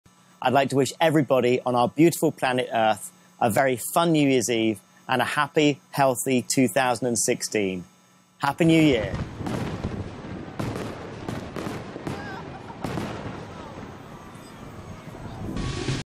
Tim Peake's New Year's Eve Message
British astronaut Tim Peake recorded a New Year's message at the International Space Station, wishing the UK a "happy and healthy 2016", and performing a back flip

and download our apps